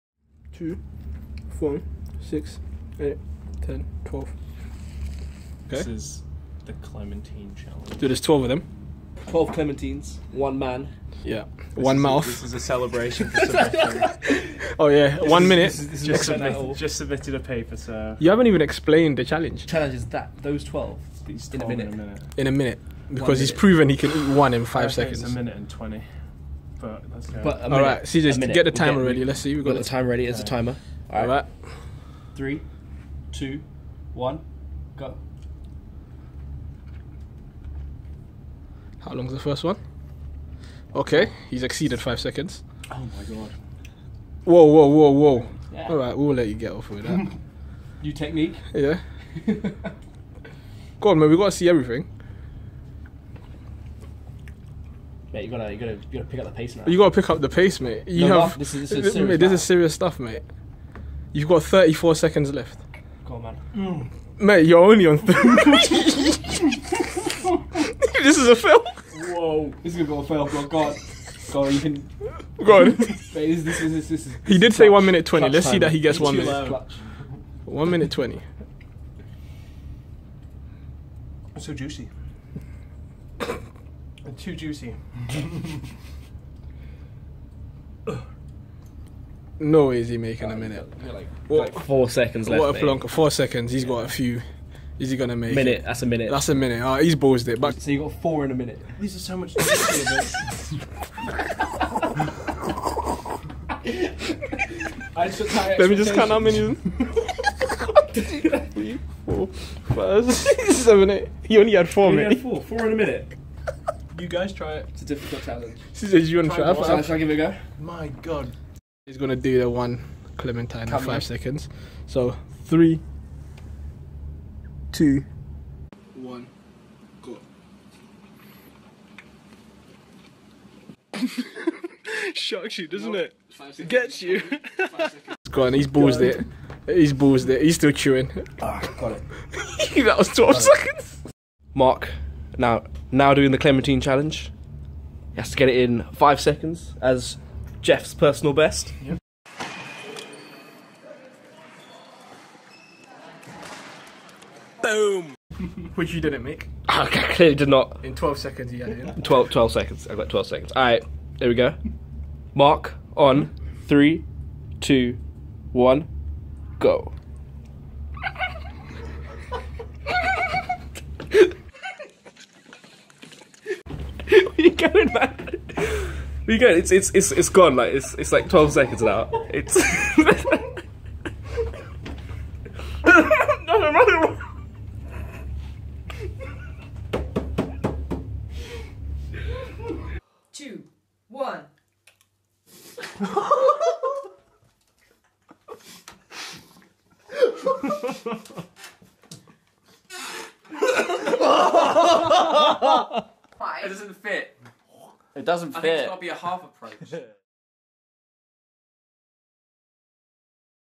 Clementine Challenge! Can you do it?
How many clementines can you destroy in a minute!? AND how fast can you kill one off!?